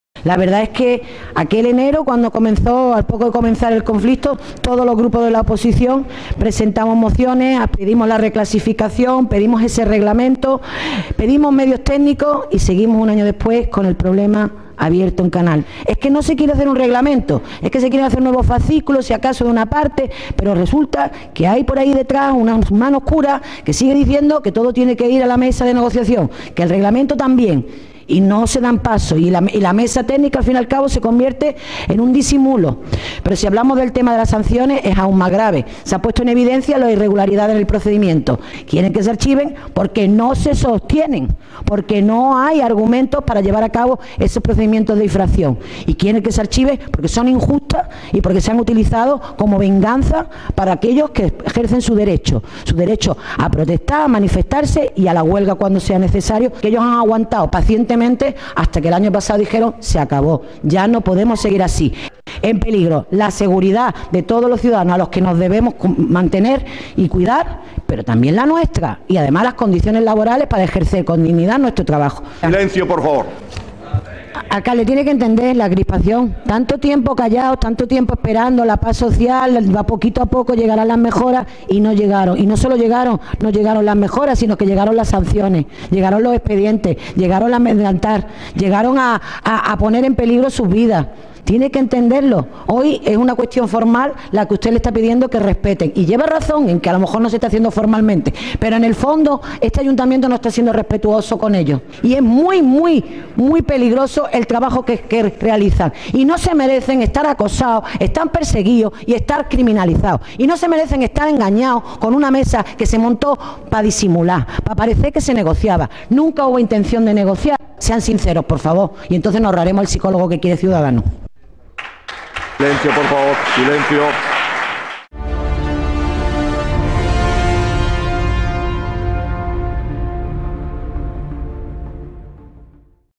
Ysabel Torralbo sobre nuestro cuerpo de bomberos
Intervención de la portavoz de Málaga Ahora en defensa del cuerpo de bomberos de Málaga ayer en el pleno